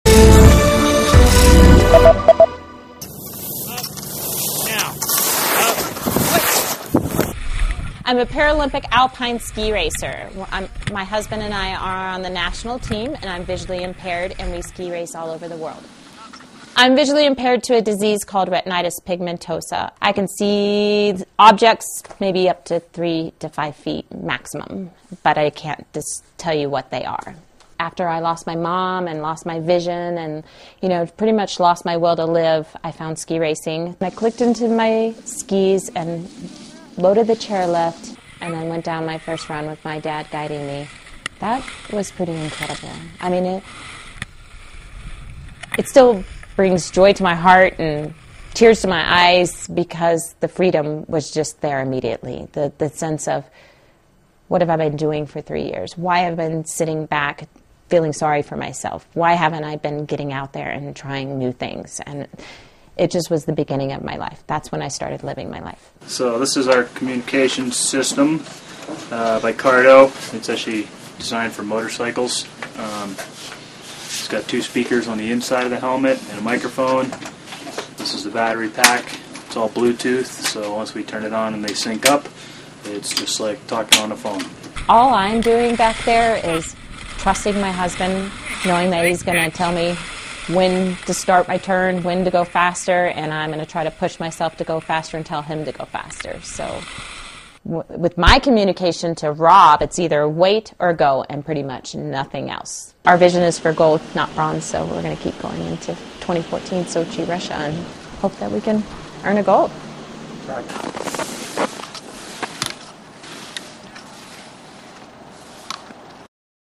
First Person: Blind Skier's Quest for Gold
U.S. Paralympic bronze medalist Danelle Umstead is visually impaired, but she has a 'vision' for gold at the 2014 Paralympic Games. Umstead and her husband communicate through a special helmet that enables them to race down the slopes together. (Jan. 31)